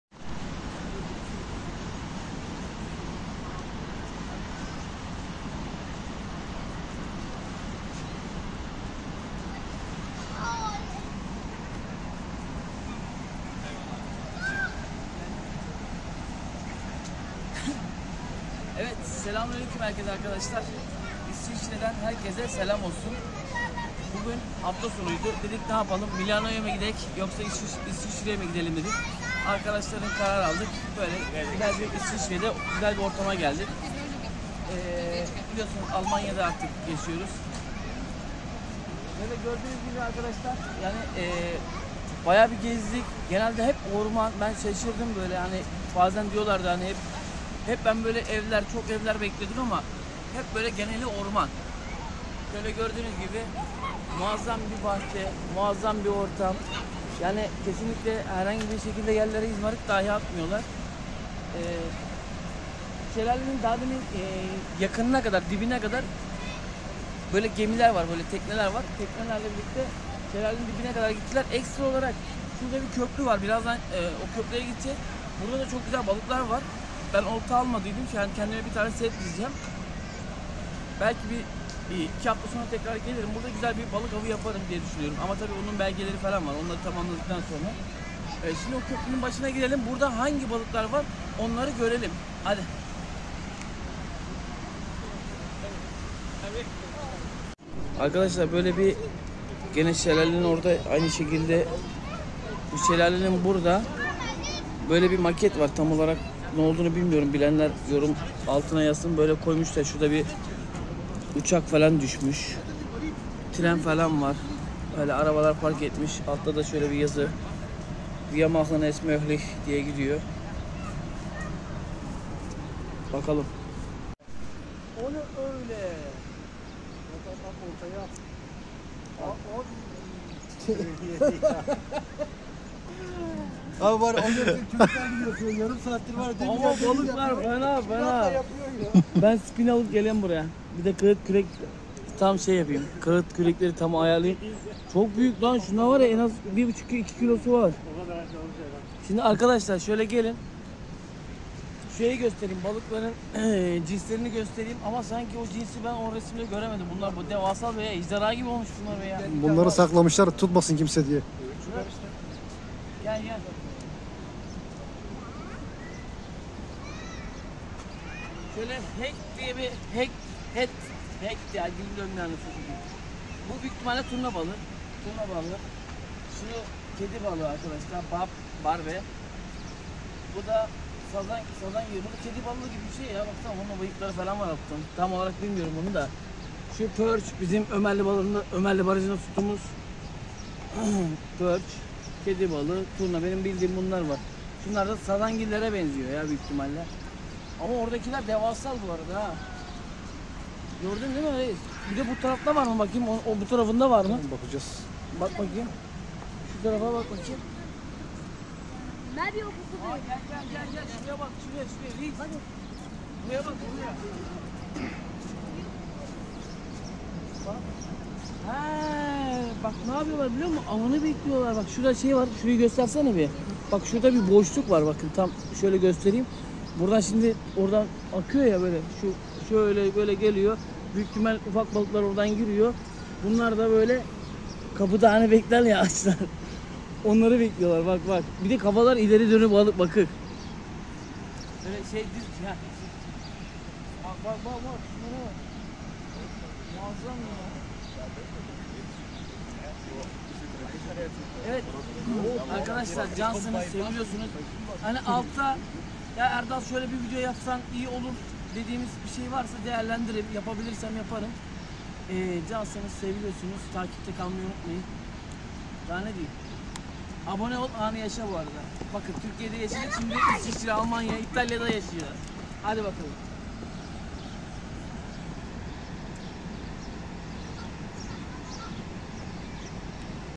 İSVİÇREDEKİ BALIKLARI GÖRÜNCE ÇOK ŞAŞIRDIM !! İSVİÇREDEKİ İLK GEZİM
Hafta sonu Almanya'dan isvicreye geçtim ve inanılmaz muazamdi . balıklar devasal